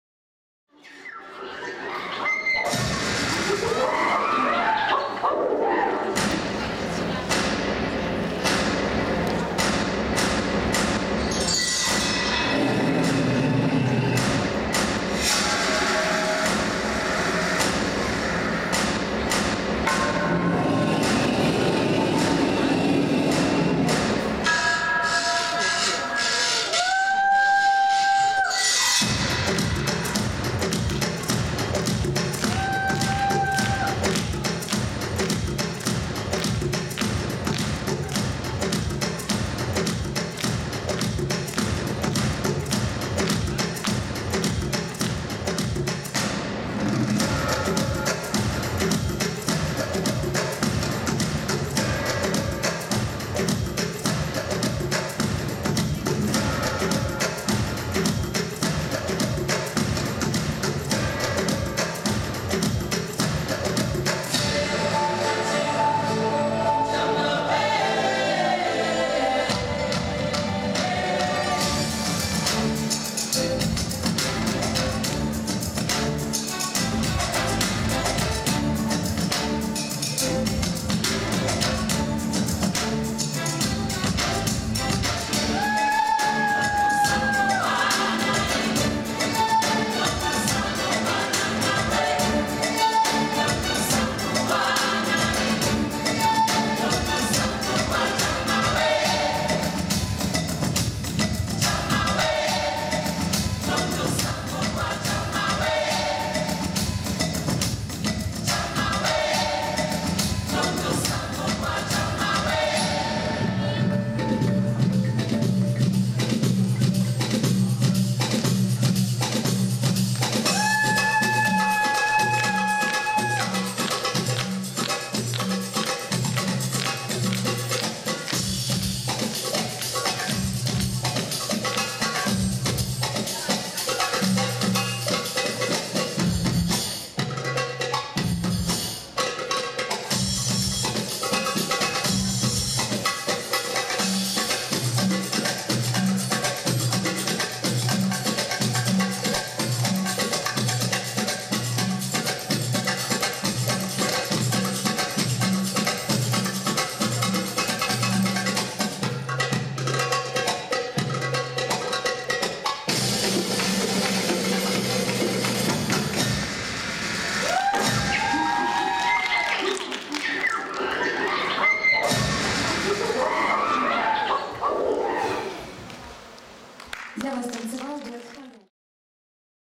«Hayati» ⊰⊱ Ukrainian Cup '14.
«Хаяти» (г.Мариуполь) на Шестом открытом Кубке Украины исполнителей восточного танца в Городском Дворце детского и юношеского творчества г. Донецк 01.02.14.
(Дуэты шоу) 66,60 .